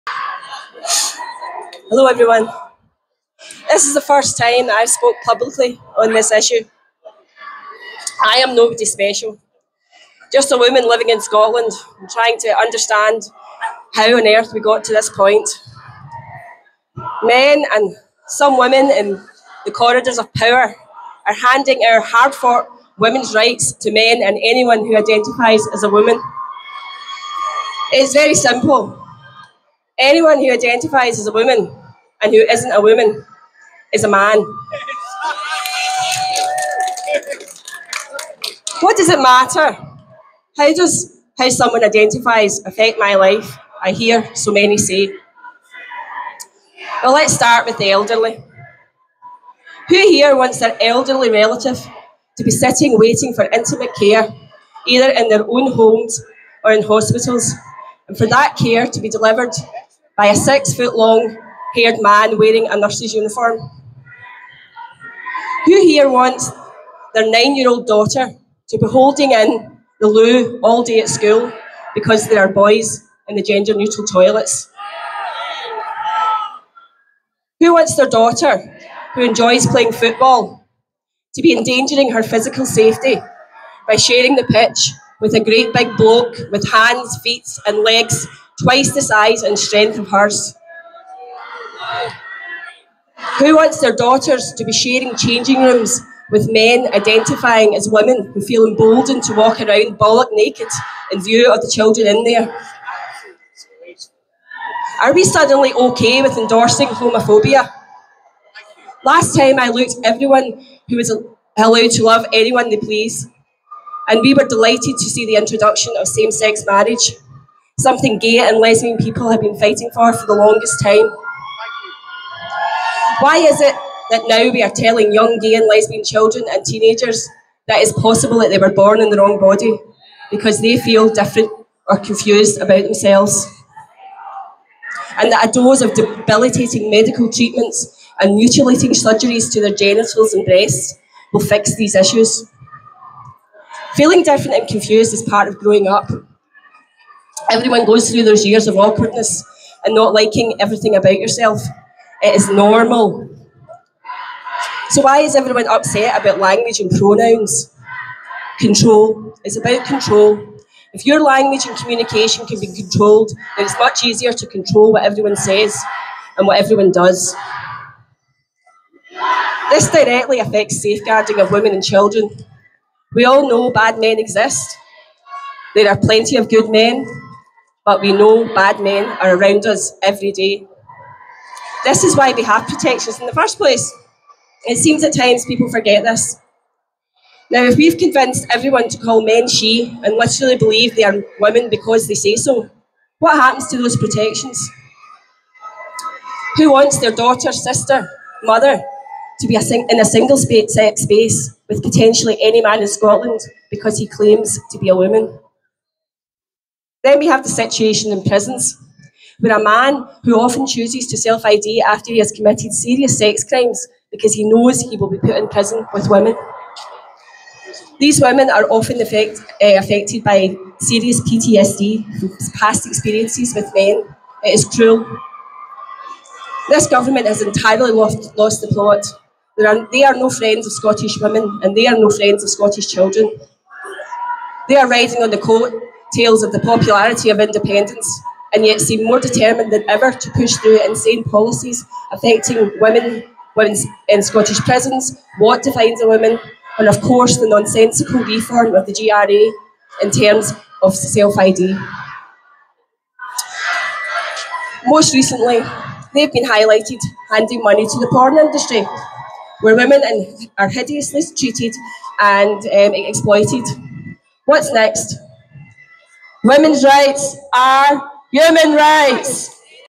Jo Dundee Won't Wheesht
Jo had never spoken publicly before about gender ideology. She highlighted safeguarding issues, women's sports and the attitude of Scottish Government towards women.